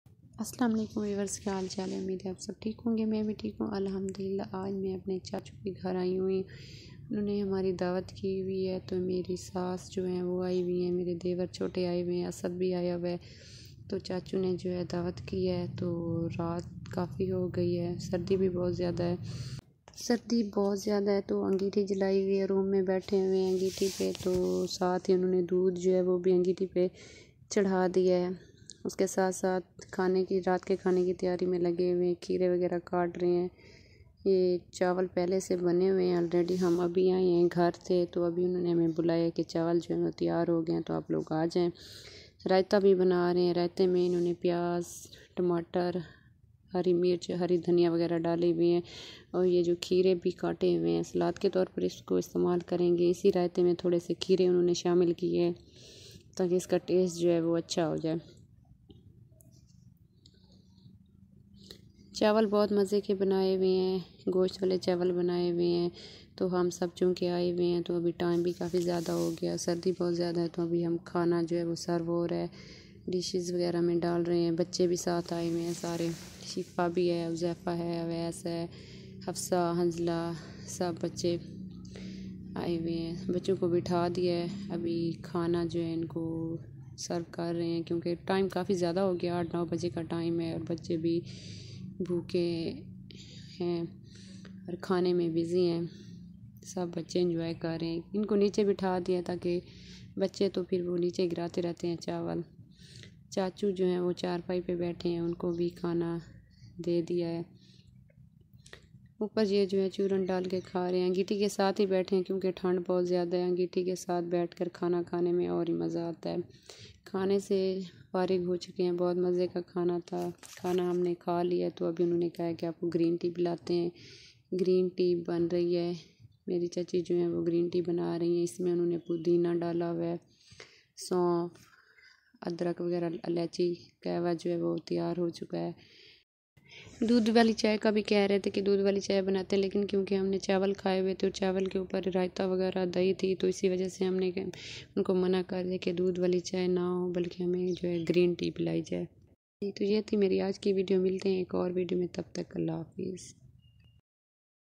aj chachu akram n khas dawat ka ahtmam kia||mahmano k liay khas pulao|| sardi m dawat
is video m main ap ko btaon gy k aj mry chachu n  hmary dawat ki aur sardi b bht thy lkn hm n bht enjoy kia